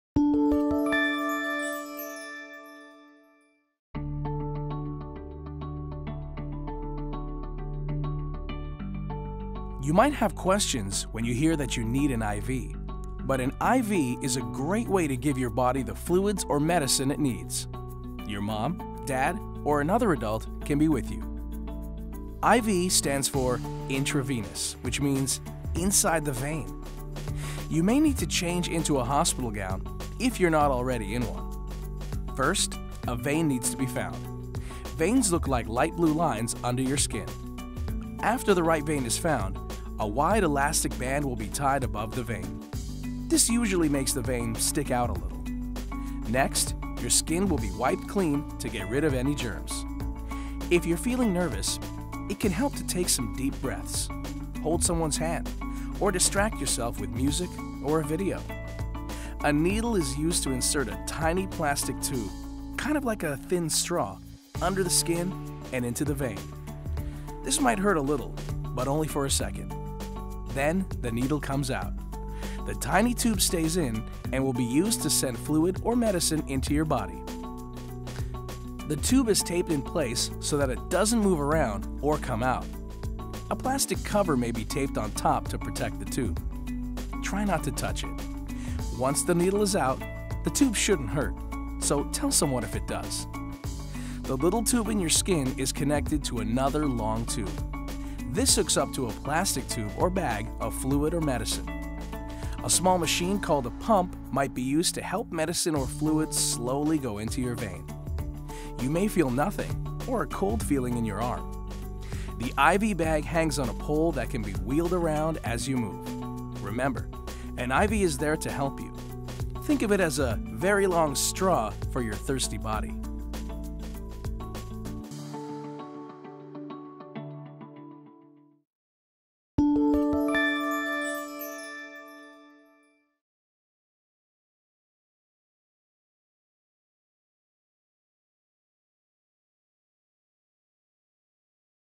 Getting an IV
IVs are a way to get fluids or medicines right into a person's body through a vein.